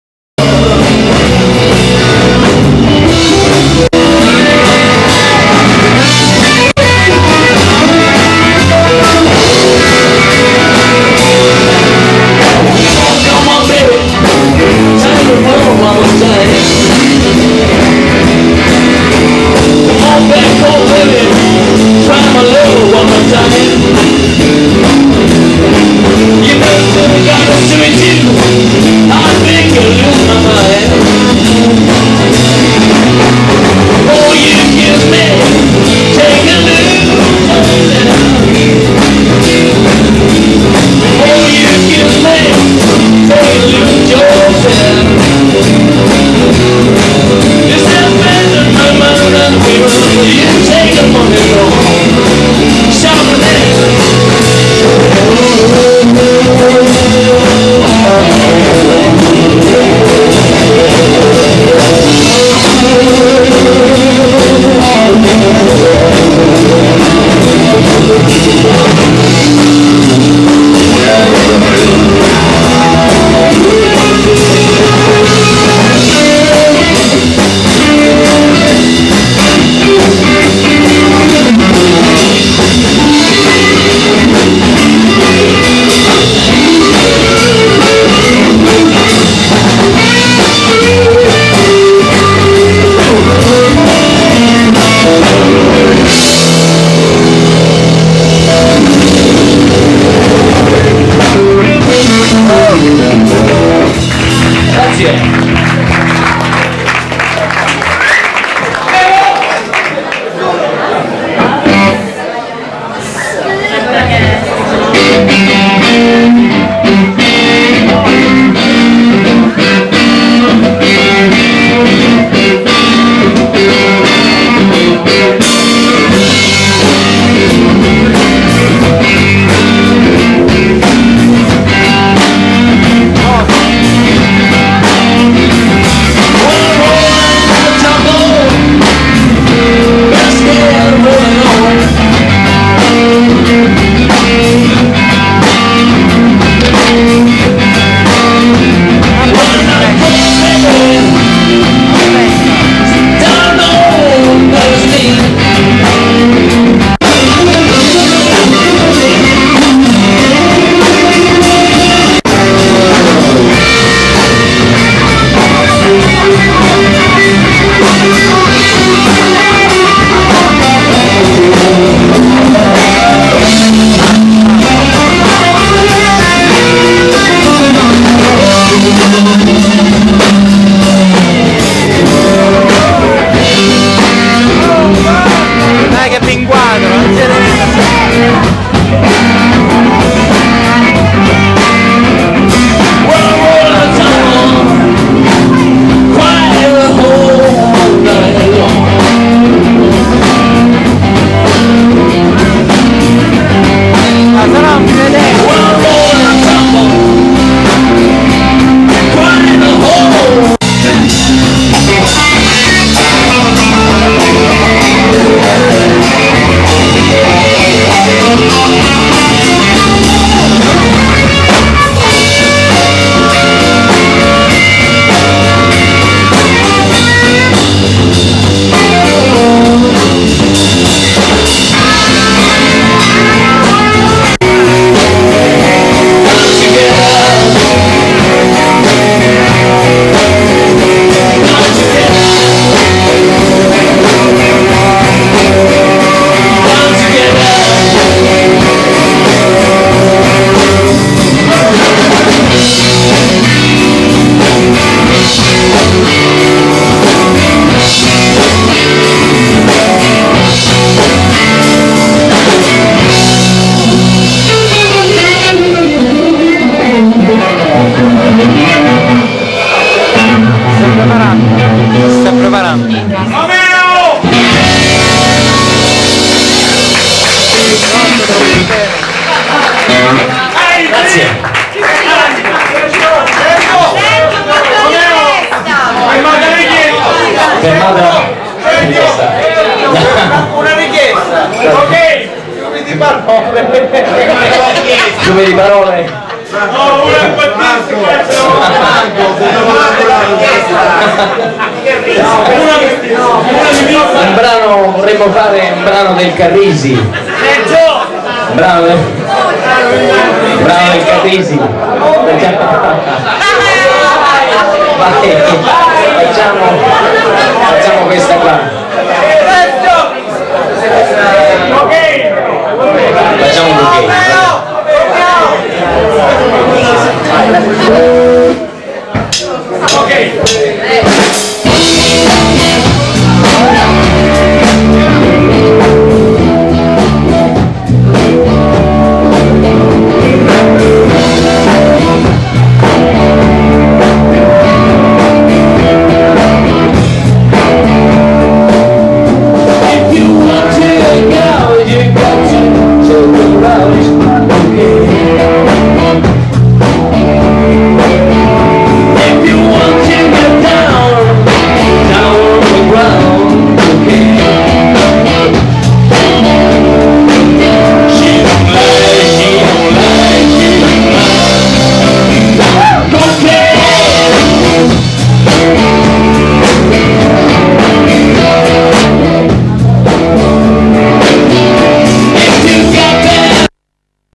POOR BOYS. Sergio Romeo, Massimo Pennisi, Emilio Catera. Via MArgutta - Acireale. 24/04/2001
POOR BOYS. Sergio Romeo: chitarra, Massimo Pennisi: Basso, Emilio Catera: batteria. Via Margutta - Acireale. 24/04/2001